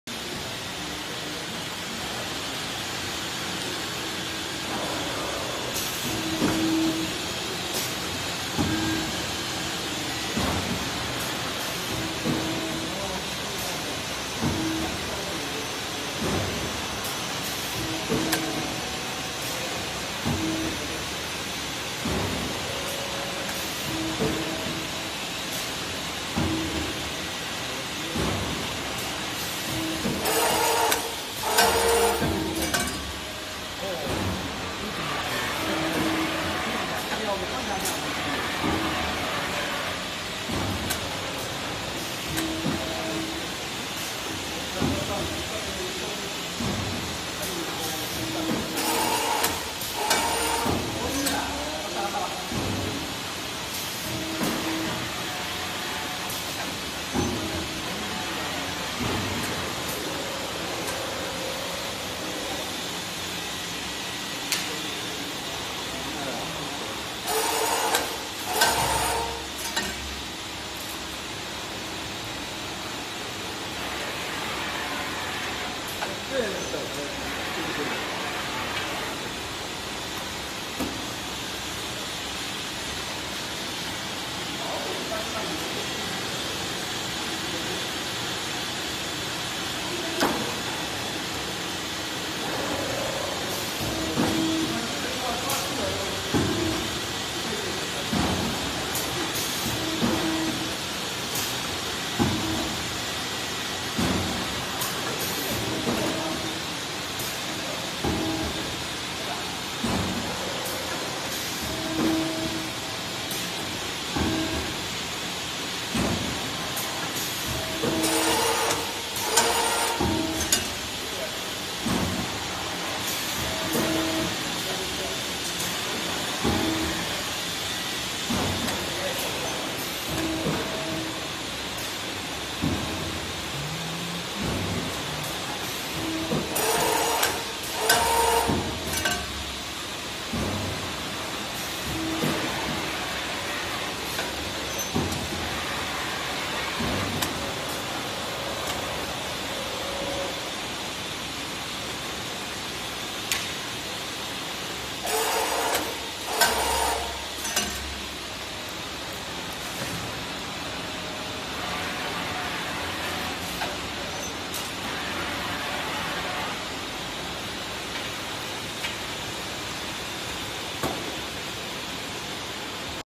MSQF-1080RTP Full Automatic Rotary Head Stripping Machine with Manipulator and Pallet Stacking
Full Automatic Rotary Head Stripping Machine with Manipulator and Pallet Stacking